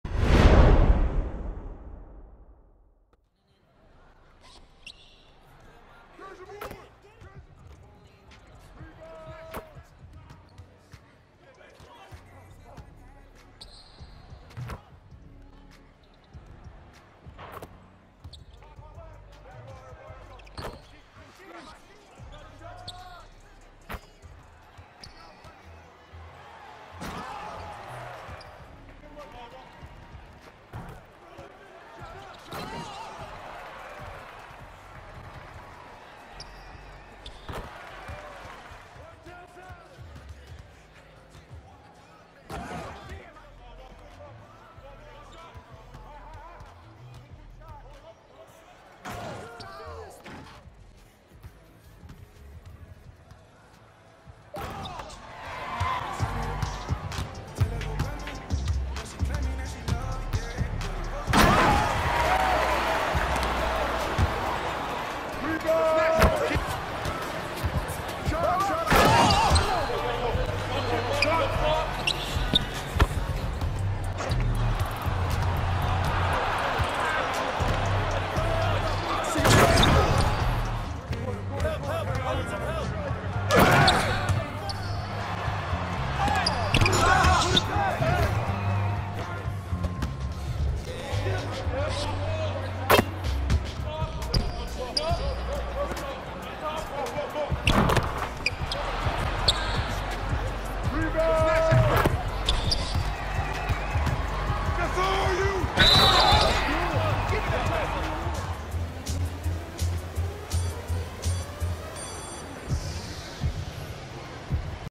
6'9 DEMI PG with CONTACT DUNKS + EASY 3'S - REC BUILD
Season 8 is here ! Some highlights of my Demigod build in the Rec !
6'9 2 Way Inside Out Scorer
20 Badges in all categories.
If you can't get 20 badges continue to level up each season to get the extra badge points.

Wassup family thanks for checking me out !
"You Feel Me Podcast" & SNR RADIO
LIVE ON TWITCH
Gaming Highlights
KEEP IT SMOOVE FOREVER